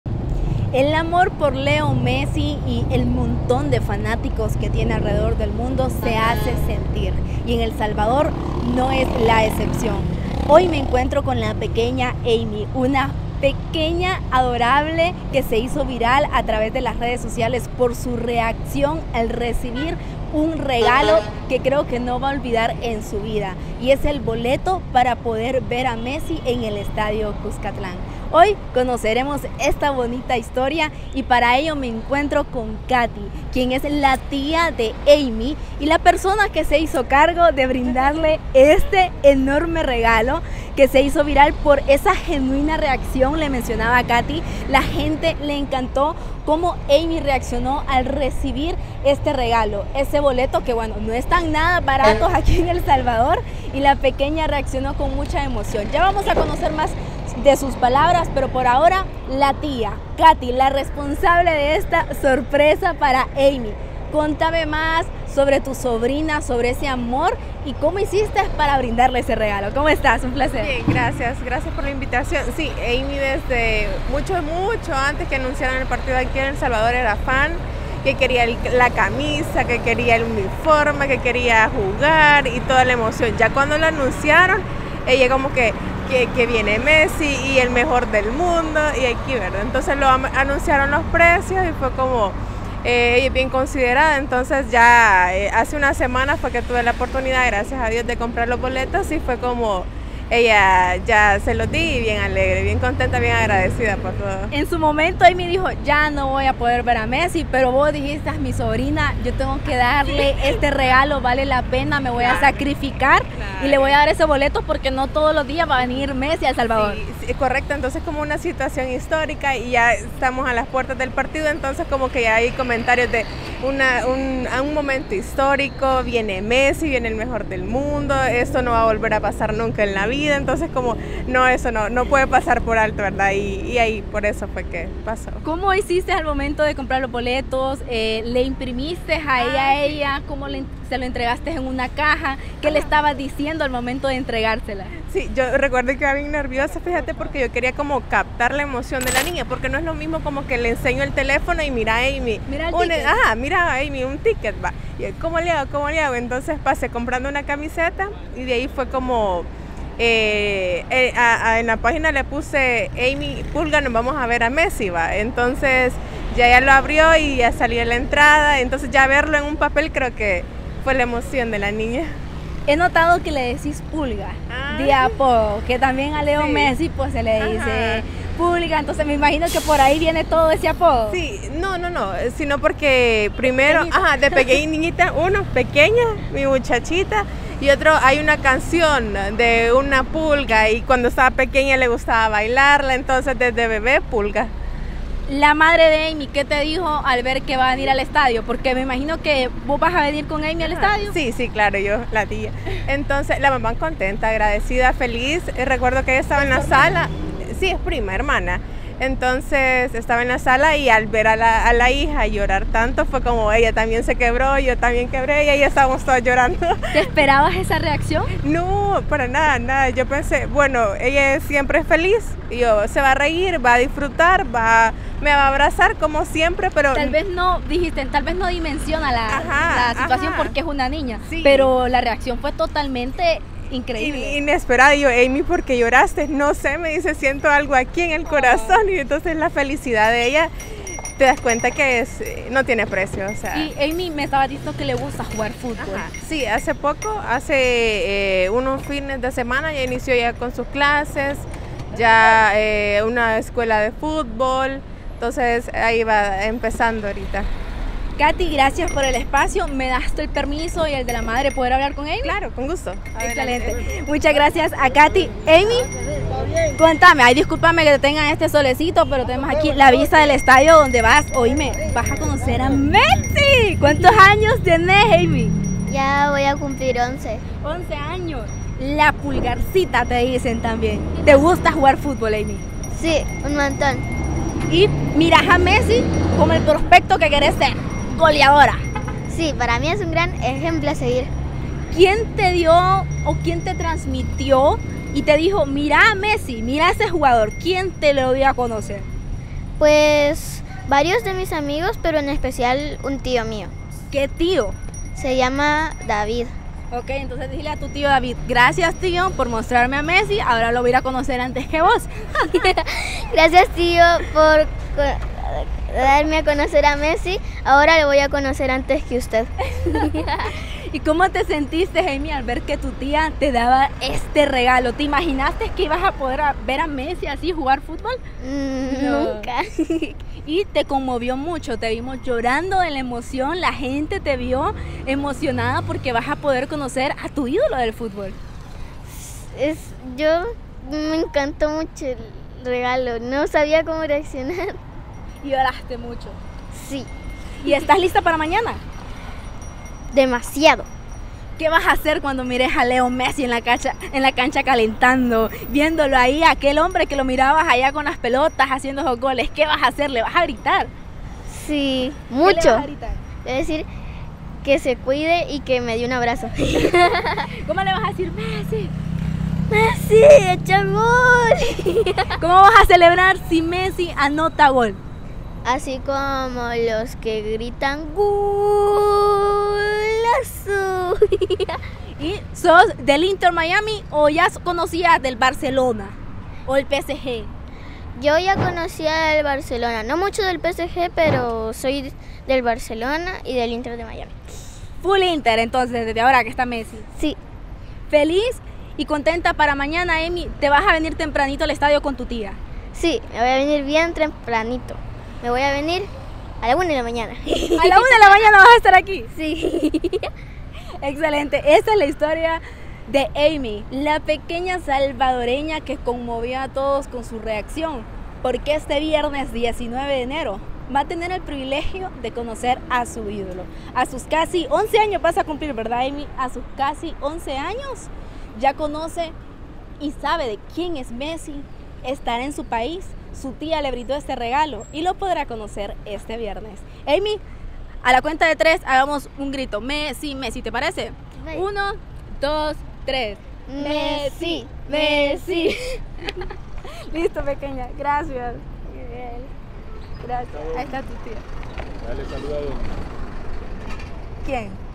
La pequeña salvadoreña que rompió en llanto al saber que vería a Lionel Messi
La pequeña Amy es una de las más grandes fanáticas de Lionel Messi, jugador que disputará un encuentro amistoso en tierras salvadoreñas.